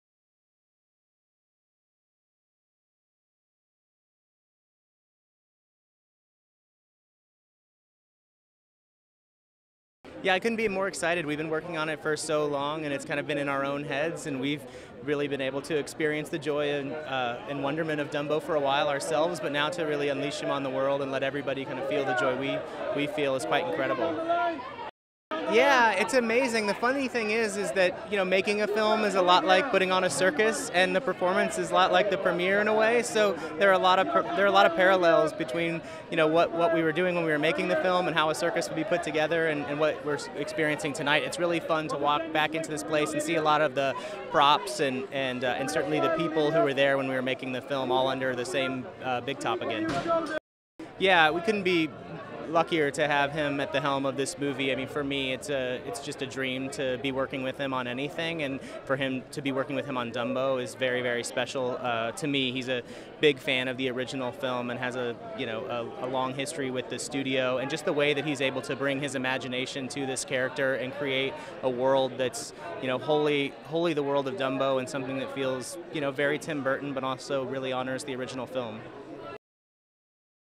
Dumbo Los Angeles World Premiere - Itw Justin Springer (official video)
Los Angeles, Hollywood, El Capitan theatre, March 11th 2019

From Disney and visionary director Tim Burton, Dumbo expands on the beloved classic story where differences are celebrated, family is cherished and dreams take flight. Dumbo opens in U.S. theaters on March 29, 2019. Circus owner Max Medici (Danny DeVito) enlists former star Holt Farrier (Colin Farrell) and his children Milly (Nico Parker) and Joe (Finley Hobbins) to care for a newborn elephant whose oversized ears make him a laughingstock in an already struggling circus. But when they discover that Dumbo can fly, the circus makes an incredible comeback, attracting persuasive entrepreneur V.A. Vandevere (Michael Keaton), who recruits the peculiar pachyderm for his newest, larger-than-life entertainment venture, Dreamland. Dumbo soars to new heights alongside a charming and spectacular aerial artist, Colette Marchant (Eva Green), until Holt learns that beneath its shiny veneer, Dreamland is full of dark secrets. The live-action reimagining of Walt Disney’s classic animated tale is directed by Tim Burton (Alice in Wonderland, Charlie and the Chocolate Factory) from a screenplay by Ehren Kruger (Ophelia, Dream House), and produced by Justin Springer (TRON: Legacy), Kruger, Katterli Frauenfelder (Miss Peregrine’s Home for Peculiar Children, Big Eyes) and Derek Frey (Miss Peregrine’s Home for Peculiar Children, Frankenweenie).

Synopsis:
Circus owner Max Medici enlists Holt Farrier to care for a newborn elephant whose oversized ears make him a laughing stock in an already struggling circus. But when Holt's children discover that Dumbo can fly, persuasive entrepreneur V. A. Vandevere and an aerial artist named Colette Marchant swoop in to make the peculiar pachyderm a star.

Dumbo
Directed by Tim Burton
Produced by Justin Springer, Ehren Kruger, Katterli Frauenfelder, Derek Frey
Screenplay by Ehren Kruger
Based on Disney's Dumbo by Otto Englander, Joe Grant, Dick Huemer and Dumbo by Helen Aberson, Harold Pearl
Starring Colin Farrell, Michael Keaton, Danny DeVito, Eva Green, Alan Arkin
Cinematography : Ben Davis
Edited by Chris Lebenzon
Production company : Walt Disney Pictures, Tim Burton Productions, Infinite Detective, Secret Machine Entertainment

(Source: Wikipedia)